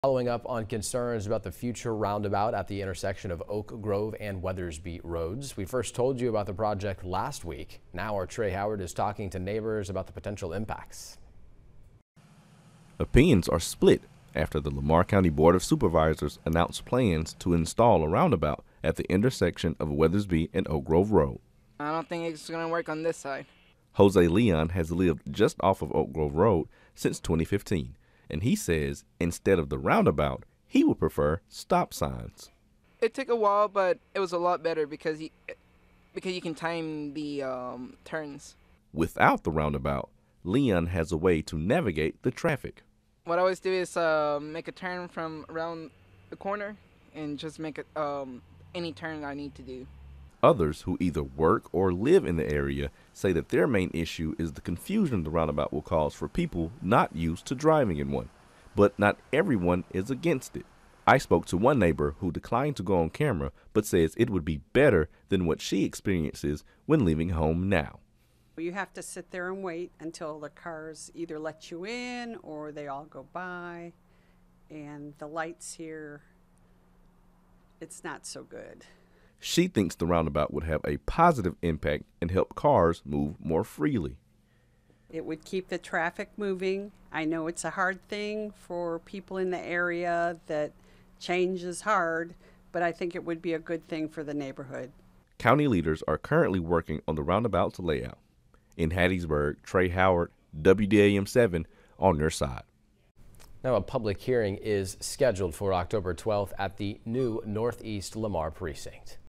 Neighbors talk new roundabout in Lamar County
Opinions are split after the Lamar County Board of Supervisors announced plans to install a roundabout at the intersection of Oak Grove and Weathersby Road.